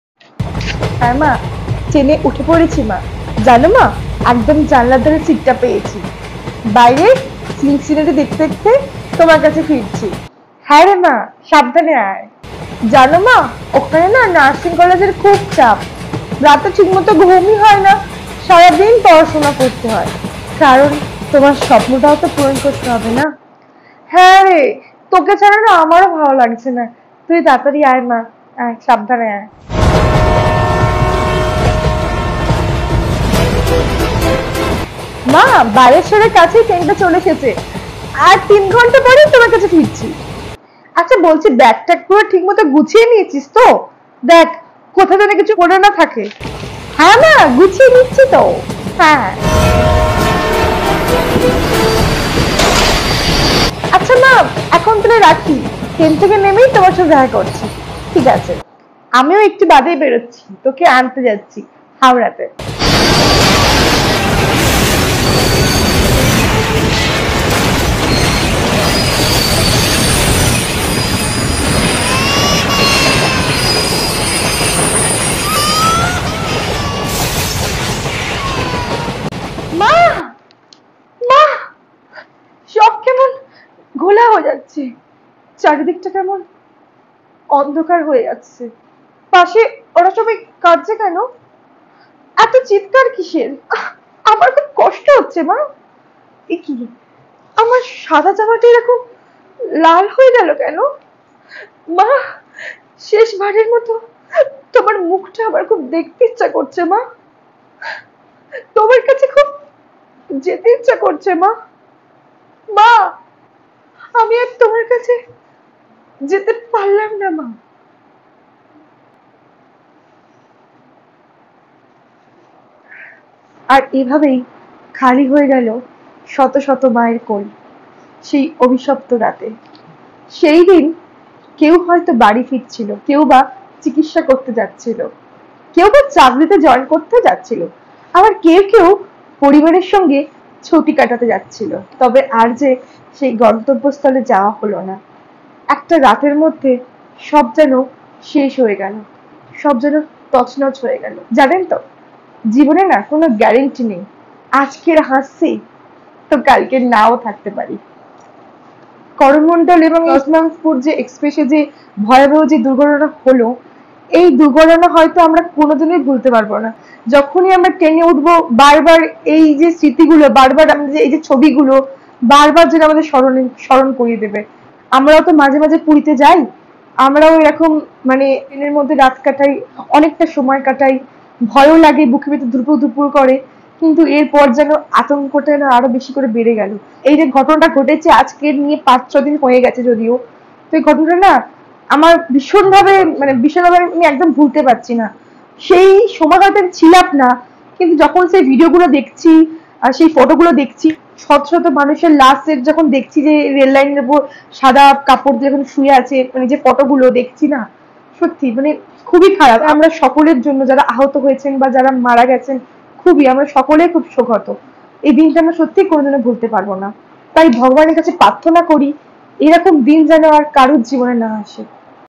ট্রেন এক্সিডেন্ট দুঃখের ভিডিও । করমন্ডল এক্সপ্রেস এবং যশবন্তপুর এক্সপ্রেস ট্রেন এক্সিডেন্টে
ট্রেন এক্সিডেন্ট দুঃখের ভিডিও । করমন্ডল এক্সপ্রেস এবং যশবন্তপুর এক্সপ্রেস ট্রেন এক্সিডেন্টে আমরা শোকাহত । মৃতদের আত্মার শান্তি কামনা করি। ভগবানের কাছে প্রার্থনা করি এরকম দিন যেন না আসে.